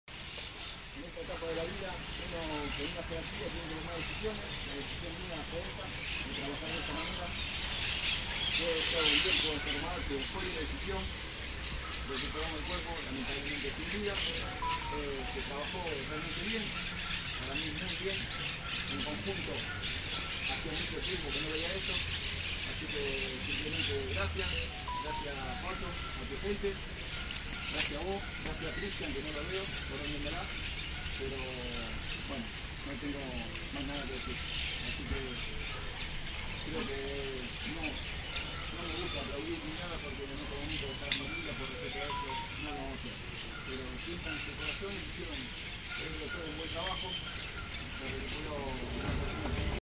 Las palabras de aliento para los rescatistas tras el operativo de rescate en la casa derrumbada
Andrés Lastorta, jefe de Bomberos Zapadores, le dio un discurso a sus compañeros tras las 40 horas de trabajo.